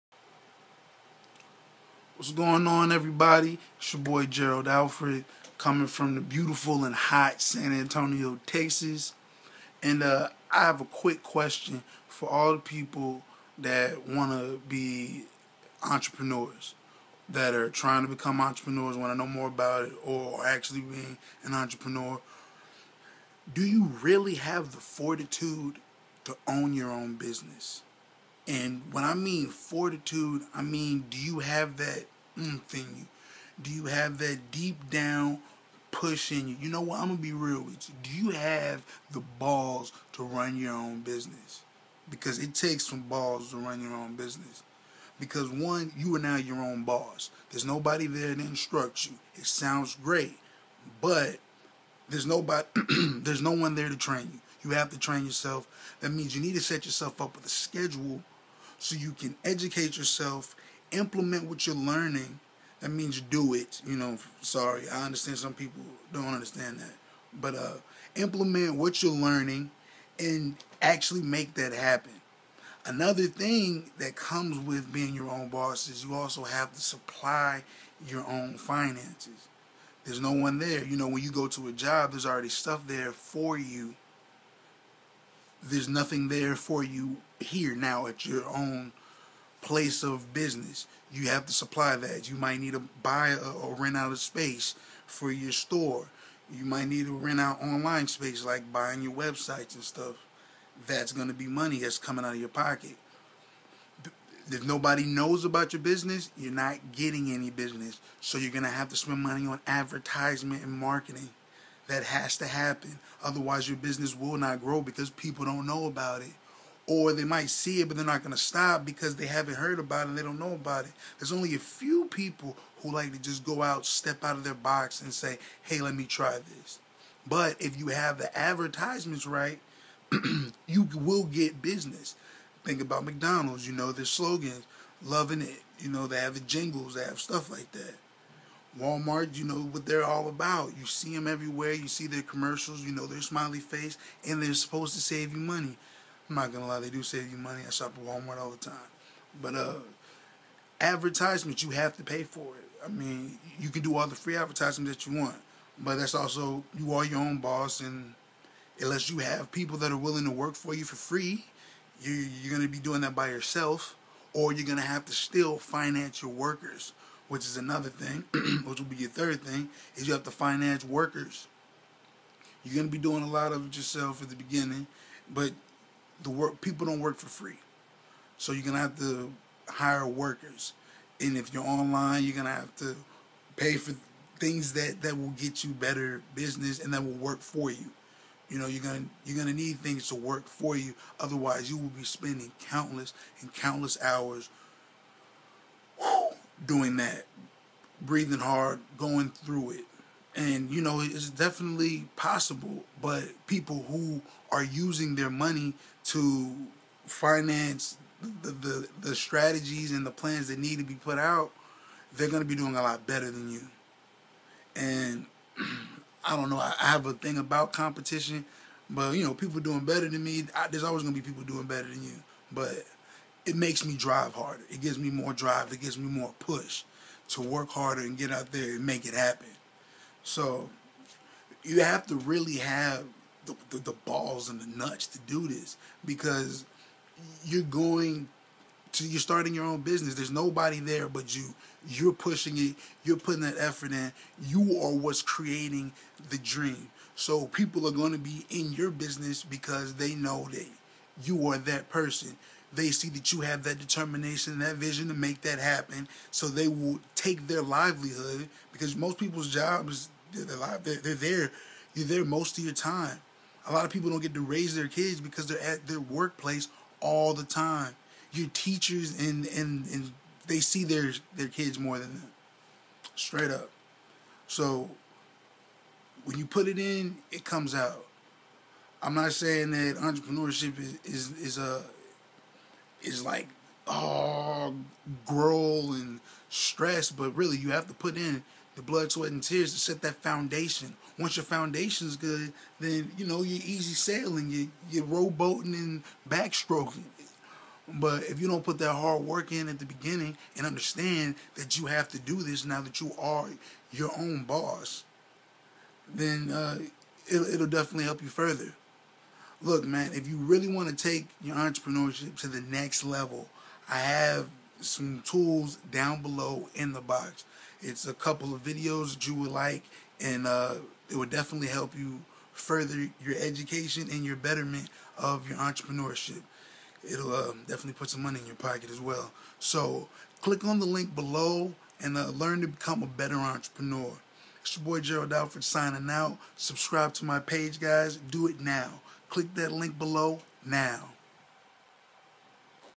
Entrepreneur Tips - Do You Have the Intestinal Fortitude to be an Entrepreneur?
- For a FREE Video Series on How to Become an Entrepreneur

Entrepreneur Tips - Do You REALLY Have what it takes to be a Entrepreneur?

Here's a huge tip for all of my fellow entrepreneurs.  Everybody wants to be their own boss and work for themselves.  Anybody who has ever had a boss has always wanted to be the boss.  People work tirelessly for somebody else and barely earn what they deserve so they decide to quit.  "That's it!  I've had it!  I'm going to work for myself"  But do you  really have the intestinal fortitude to be an Entrepreneur?  Do you really have that inner drive to strive for what you actually want?  Let me be more frank about this: Do you really have the balls to run your own business?

The thought of being an entrepreneur is great and more power to you but honestly, do you have what it takes. Truthfully being your own boss is more like a huge dream! You picture yourself making great money, owning your whatever you want to own (lol), spending more time with your family, you know, living life the way it's supposed to be lived! And I'm not saying that can't or won't happen because it will. if you really have the fortitude to make it.

I'm not going to be the person that's going to lie to you.  In fact, I'm going to be as honest as I possibly can.  It's not as easy as it seems to be an entrepreneur.  First of all, you no longer have anyone telling you what to do, so you have to take responsibility for yourself and really PUSH!  You're going to need to set yourself up a schedule and STICK TO IT!

Now here's the tough part.  The finances.  Do you really have the balls to invest in your dream and hold steady to that investment EVEN in times when business is slow or non-existent.  Especially at the beginning.  You're going to be dishing out a lot more capital than you'll be receiving.  Can you take that?  Can you handle investing your hard earned money into your dreams?

Since you're the boss, all expenses for YOUR business must come out of YOUR pockets. Can you do that? And I don't mean for like a month a two. If you've really made the decision to be an entrepreneur, you are going to HAVE TO stick to it longer than a few months. This is your life we're talking about! Do you want it or not? This is what you actually should work for. Take your hard eanred money from your J-O-B and use it to finance your dream. That way when the time comes. BOOM! You can fire your boss and BE YOUR OWN BOSS!

Everyone likes the idea of being an Entrepreneur but here's the deal.  You actually have to put the work in.  You actually have to finance it.  And finally, you actually have to stick with it.  If you don't have the fortitude for that, it's cool.  Keep working for "The Man" the rest of life.  Then try to live off that retirement check.  Sounds great right?  If you're ready to be an Entrepreneur and take full responsibility for your success click here Now: Start Living Your Dreams Today!